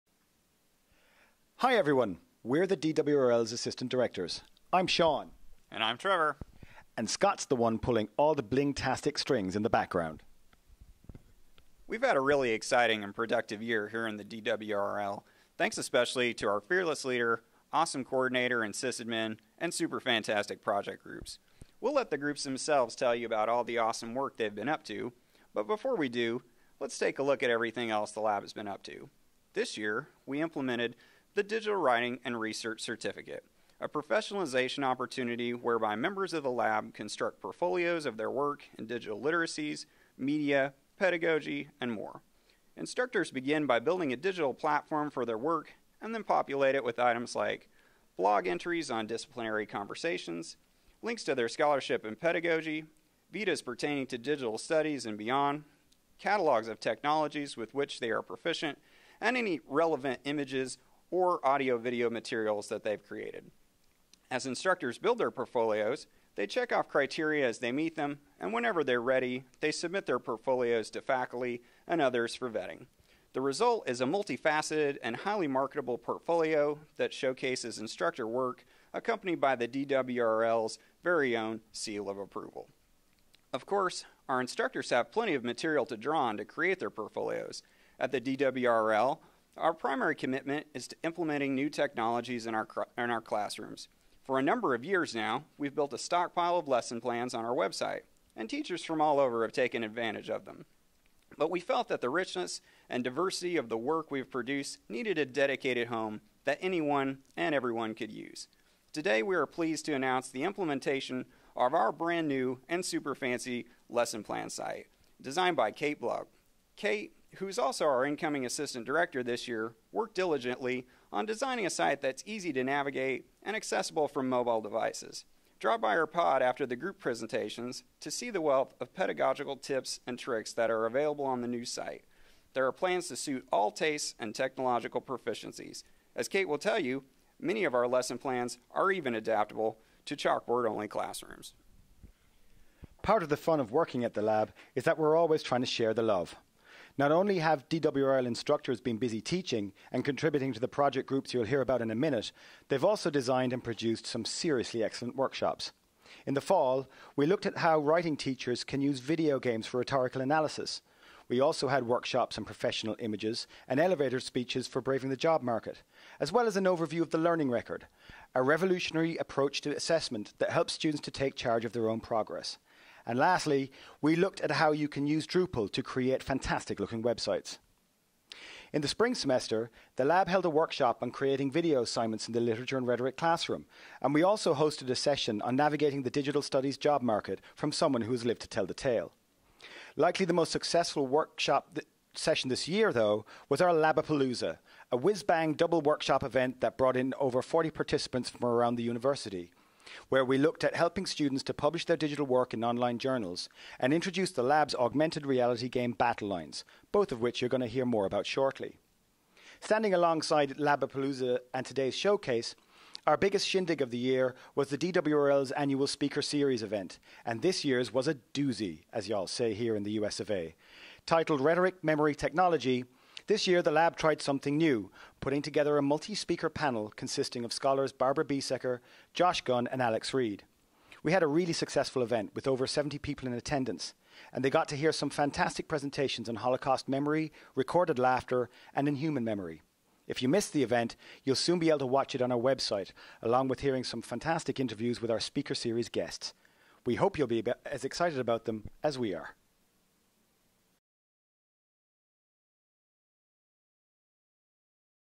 2012 DWRL Showcase (Assistant Directors' Overview)
This is the introduction to the Digital Writing and Research Lab's 2012 Showcase. The Assistant Directors outline the work done over the past year in the lab.